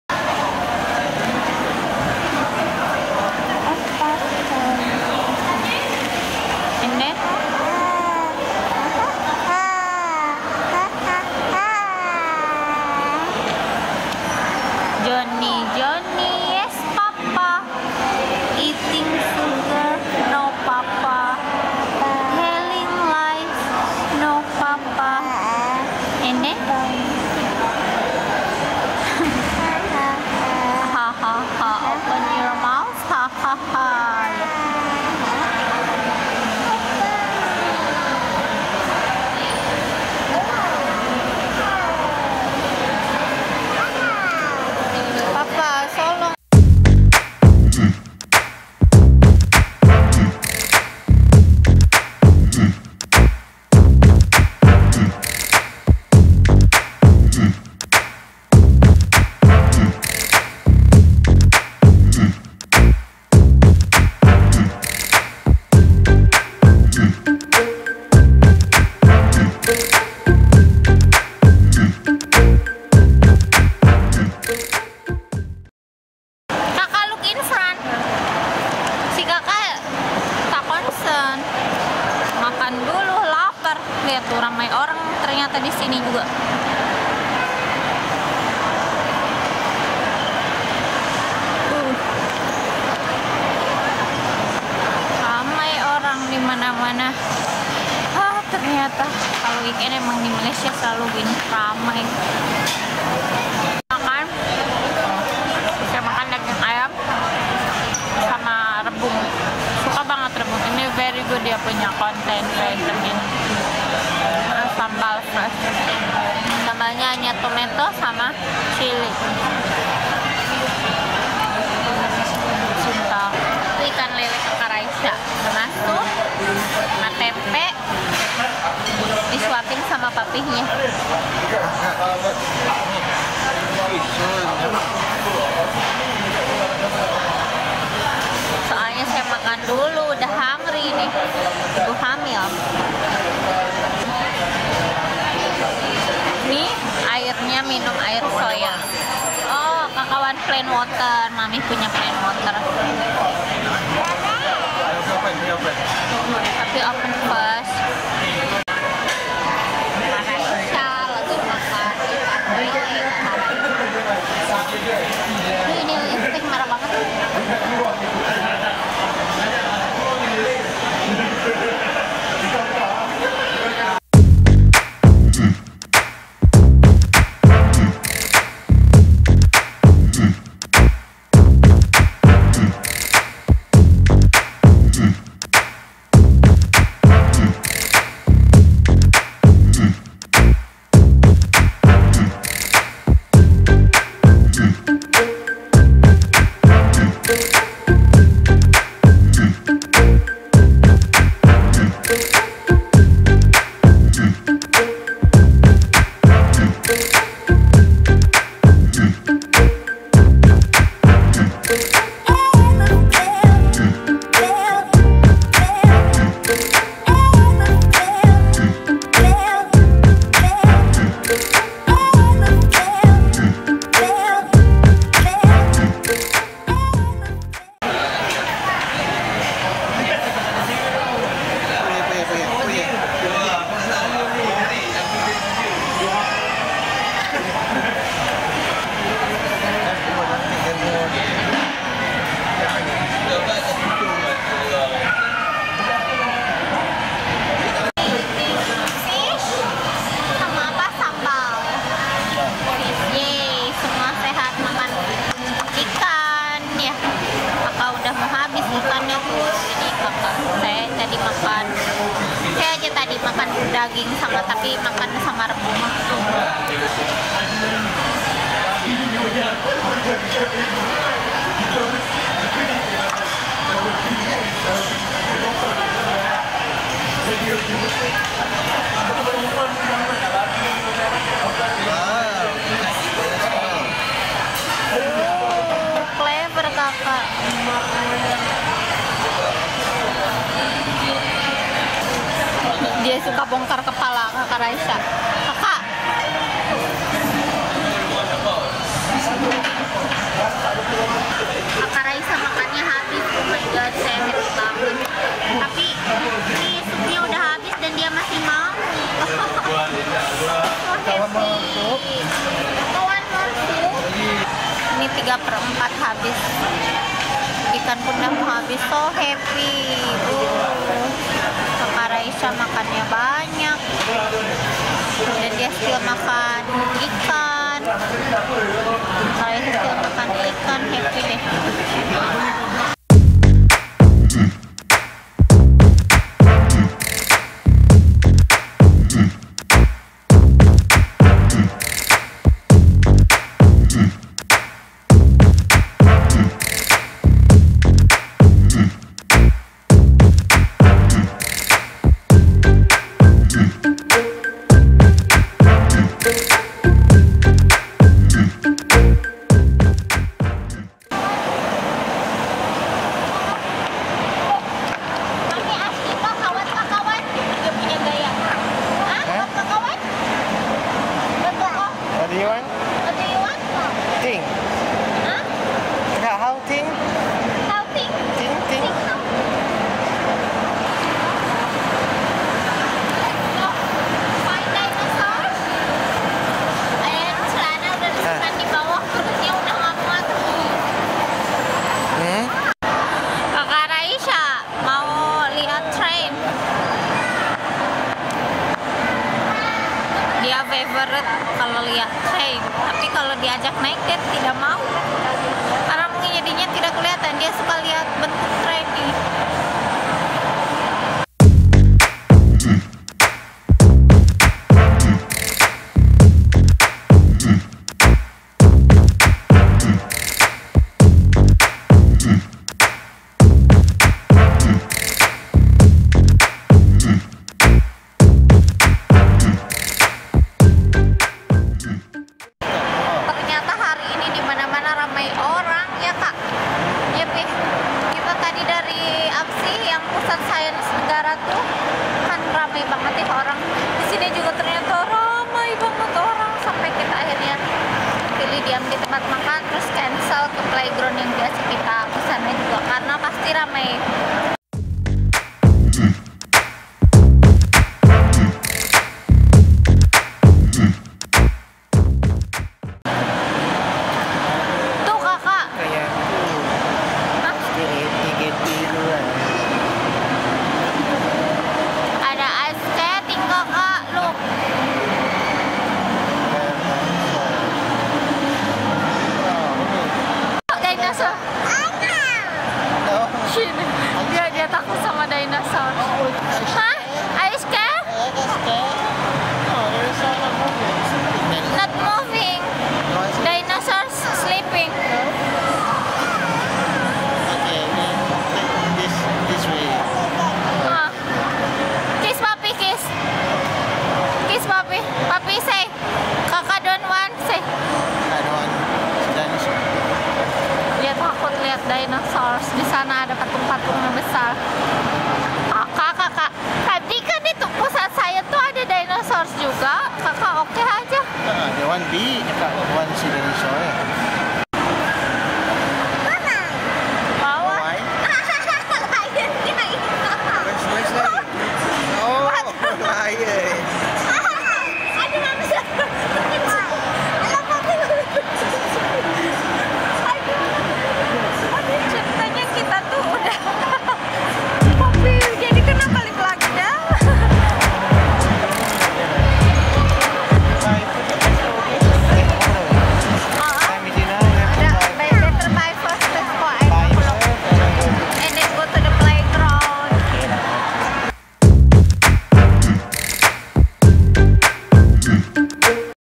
Makanan Favorite Raisha di Malaysia | Food Junction IOI city Mall| Righaz Family Vlog
Makanan Favorite Raisha di Malaysia | Righaz Family Vlog

Hari ini saya ingin shoot moment dimana anak saya selalu makan lahap makanan favoritenya. Makan apa yang buat kk Raisha sampai makan lahap, Jom lihat videonya ;)